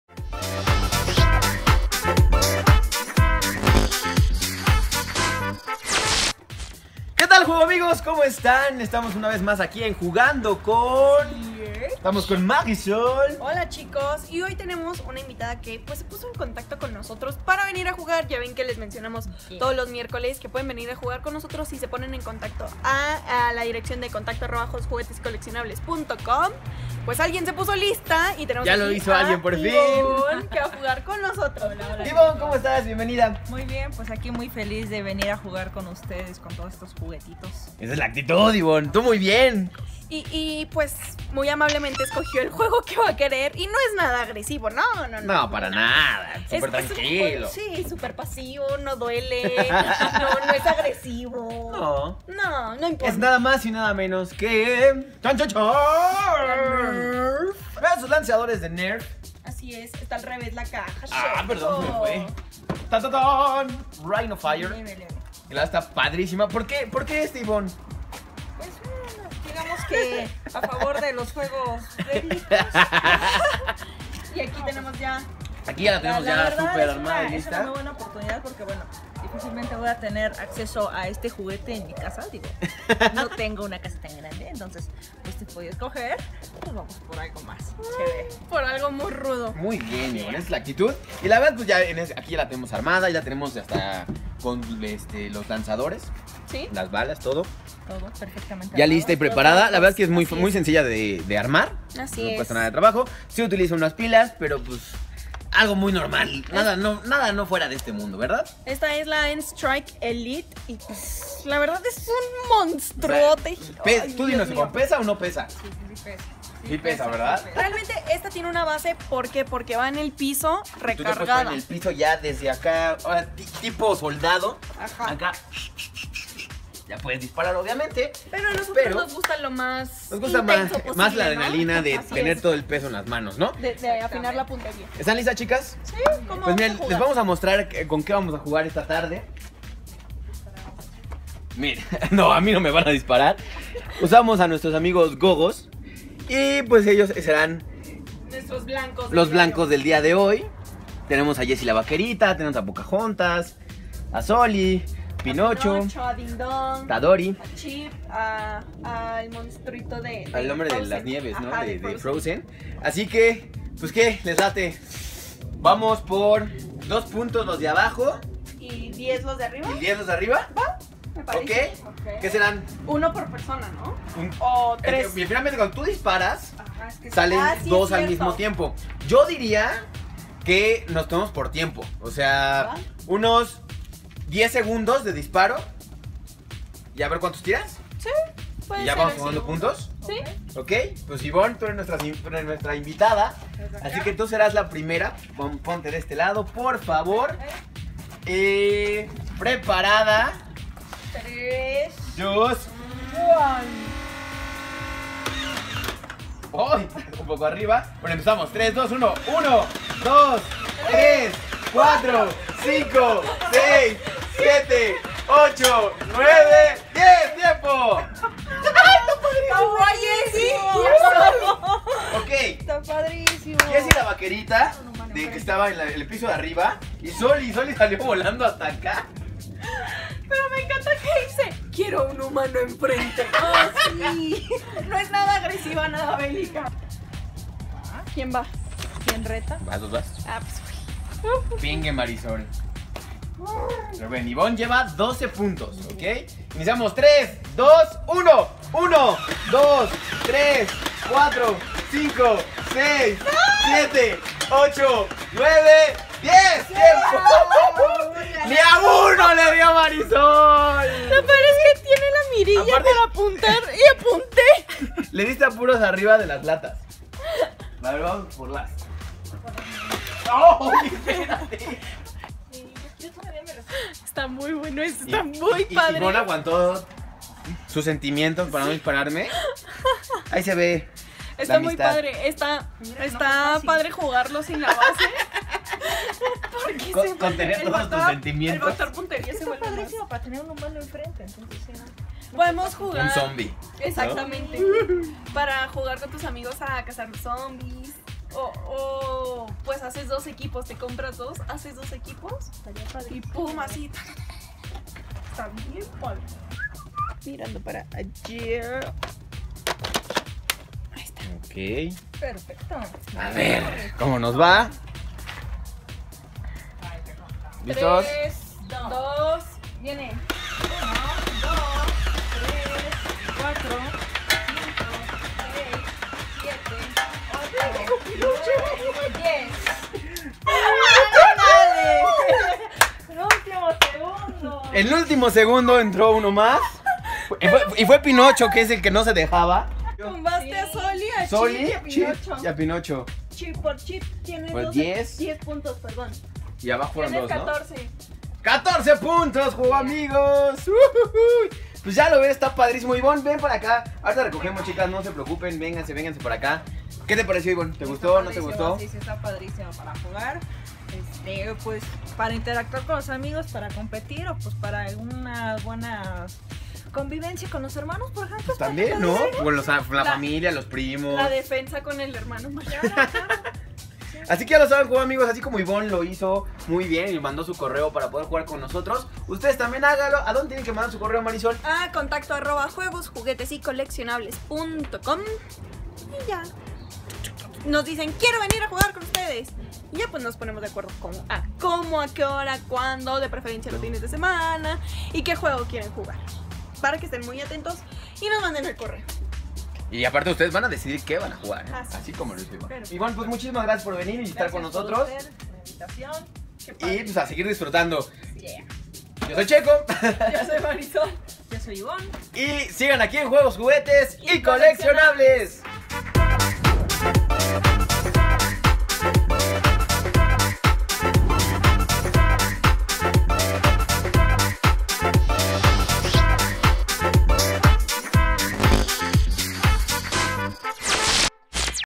Jugando con NERF N-Strike Elite invitada juegos juguetes y coleccionables
Es la Guerra con NERF N-Strike Elite Rhino-Fire Blaster ¿Quien sera el Ganador?